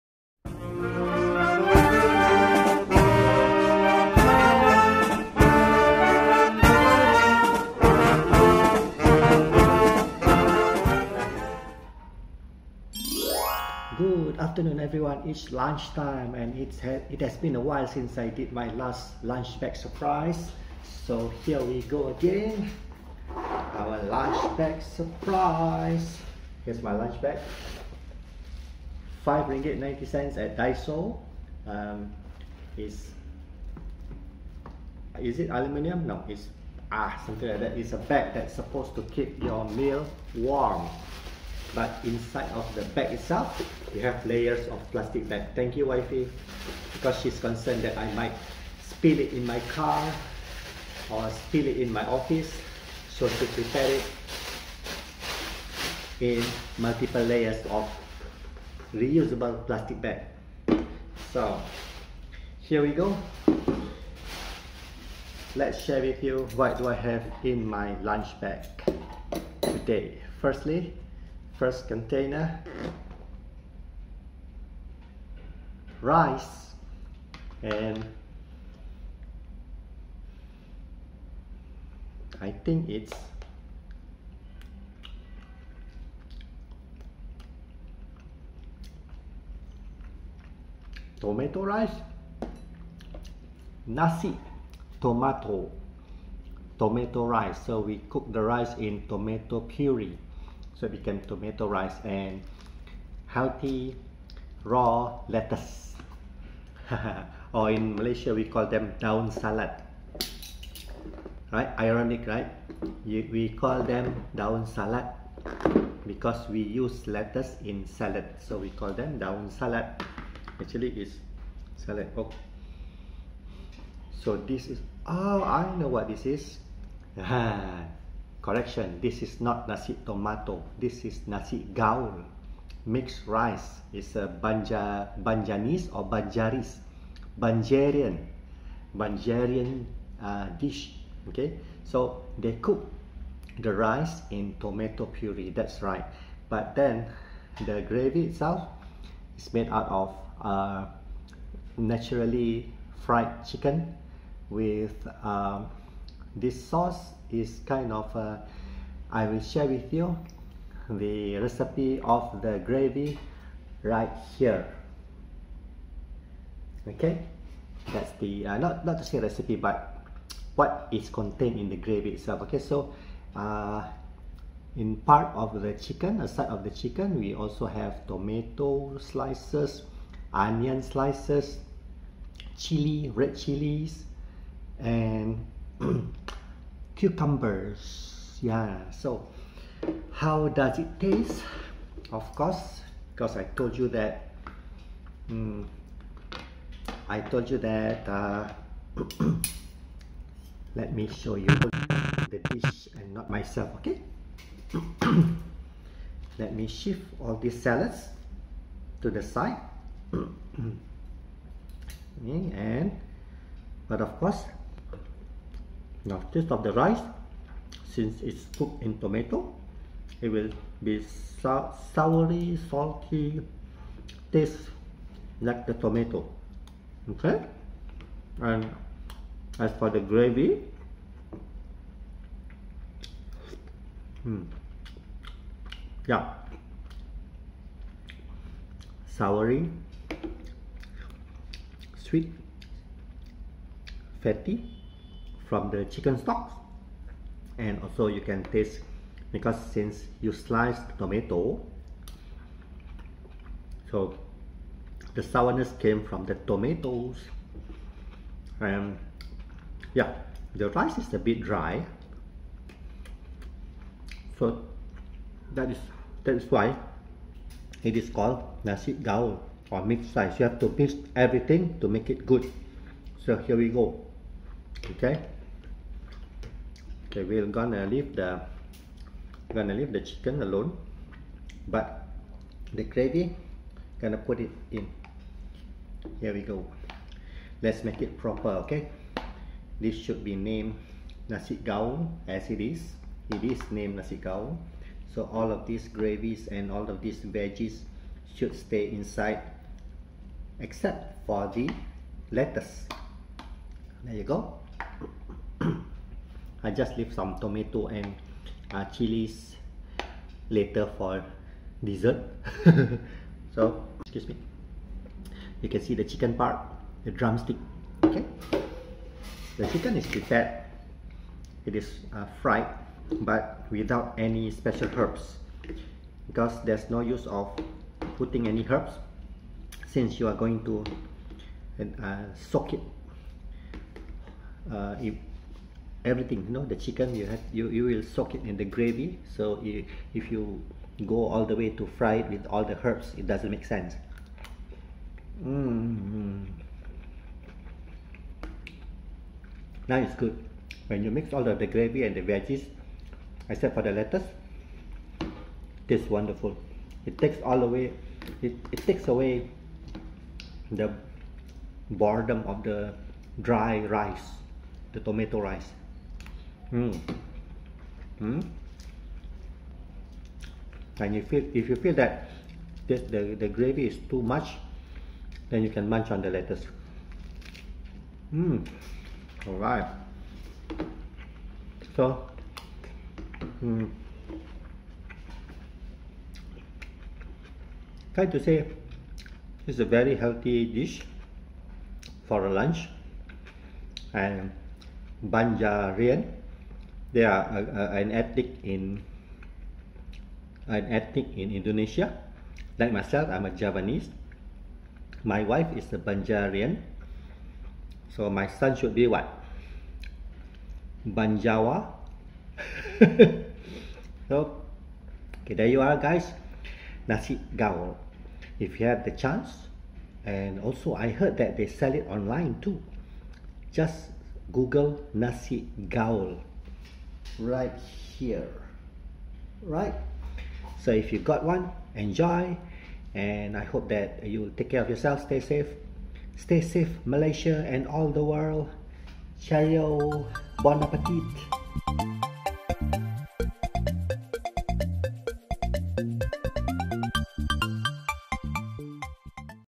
Lunch bag surprise ; Banjarian Nasi Gaul
Today's lunch is the famous Banjarian rice dish, the Nasi Gaul